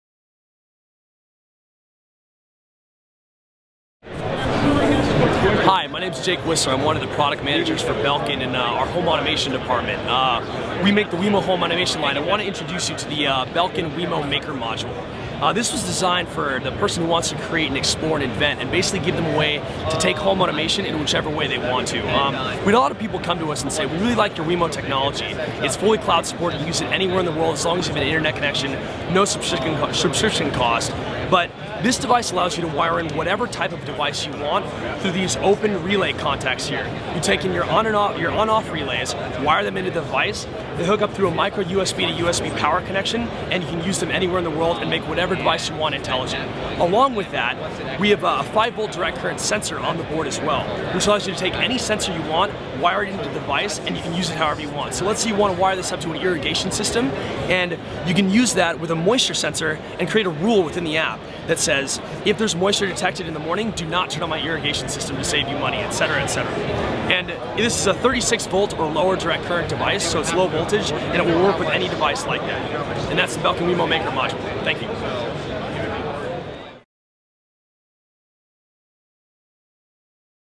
Belkin's WeMo Maker at CES 2014
At International CES 2014, Belkin's WeMo platform of connected home products grew to include a hacker-friendly module called the WeMo Maker. The device allows you to switch up to 36 volts DC or take readings from 5 volt analog sensors. All of this can done remotely through the Internet with Belkin's app via their cloud service. For local control, you can use the WeMo SDK to roll your own service if you prefer. In the video above, I got a quick overview of the WeMo Maker from the Jake Whisler, a Belkin Product Manager. They expect that the device will be available this summer. No pricing information is available yet.